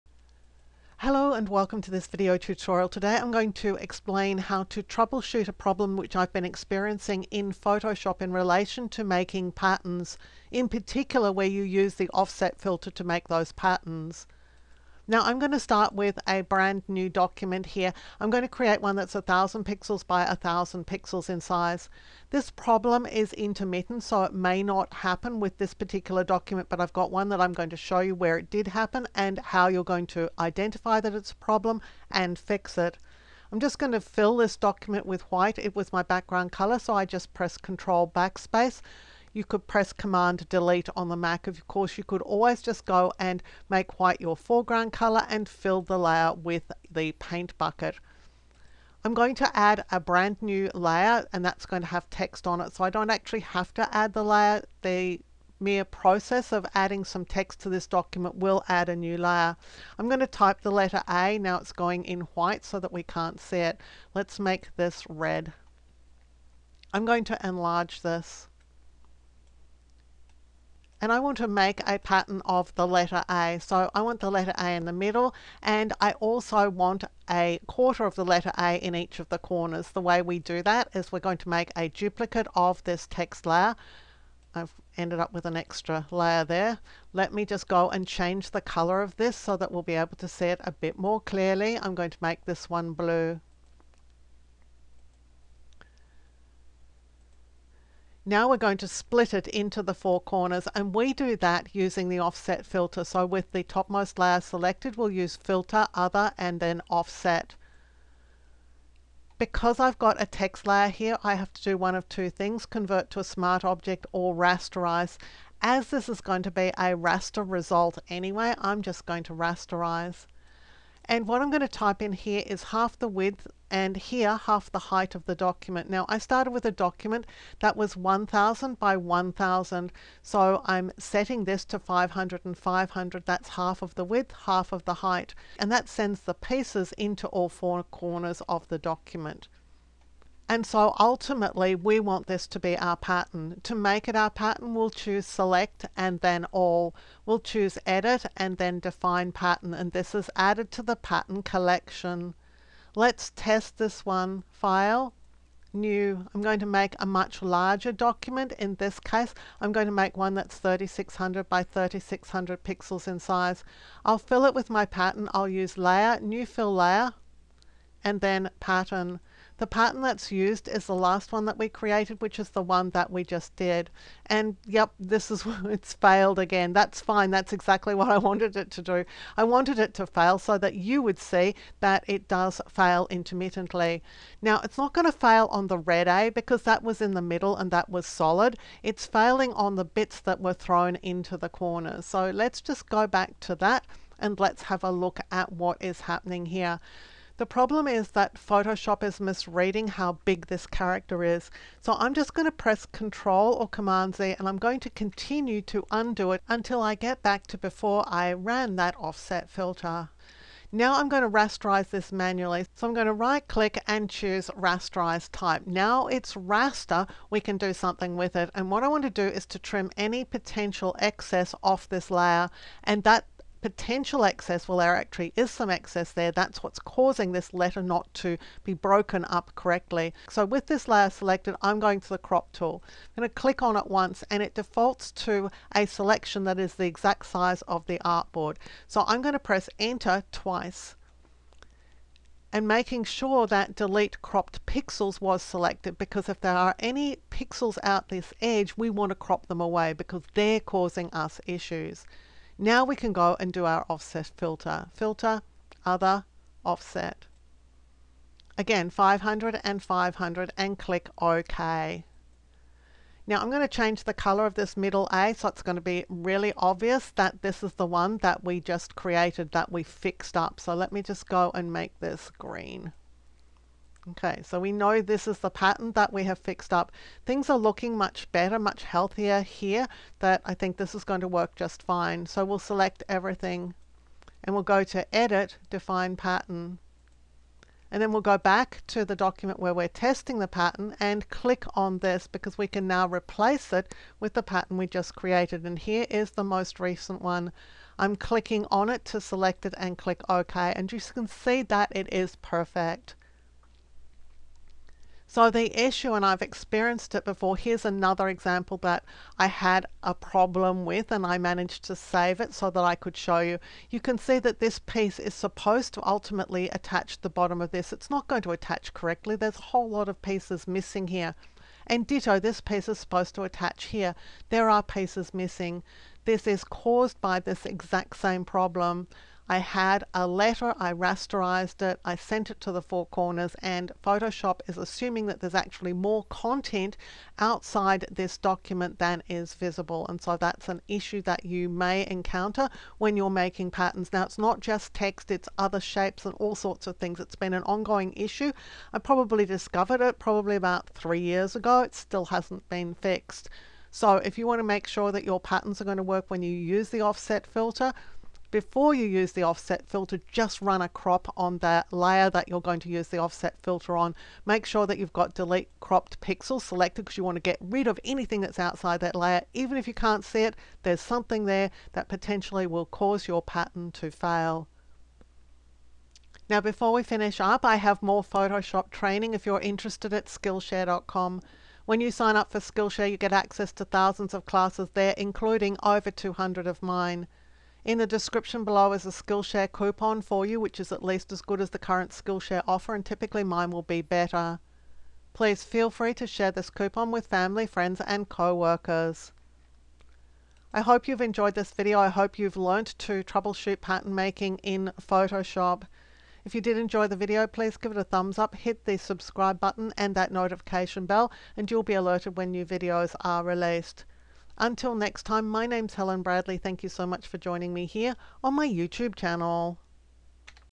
Photoshop - Troubleshoot Pattern Problems - Offset Filter Failure
How to fix problems with your Photoshop patterns failing when you use the Offset filter to make them. This is an intermittent problem with the most recent versions of Photoshop and I have a simple solution to this that is easy to implement.